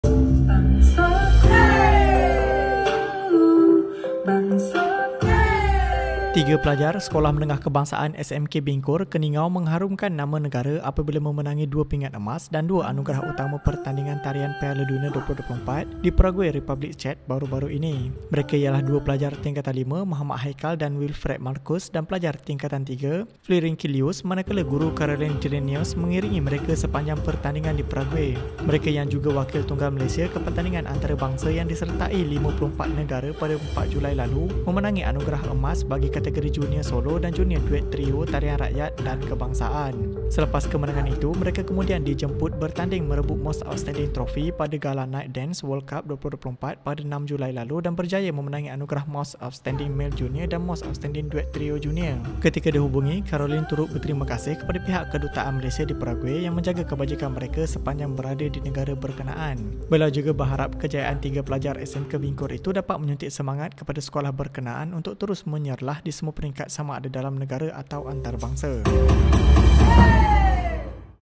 Pelajar SMK Bingkor, Keningau juara pertandingan Piala Dunia Tarian 2024
KOTA KINABALU: Tiga pelajar Sekolah Menengah Kebangsaan (SMK) Bingkor, Keningau mengharumkan nama negara apabila memenangi dua pingat emas dan dua anugerah utama pertandingan Tarian Piala Dunia 2024 di Prague, Republik Czech, baru-baru ini.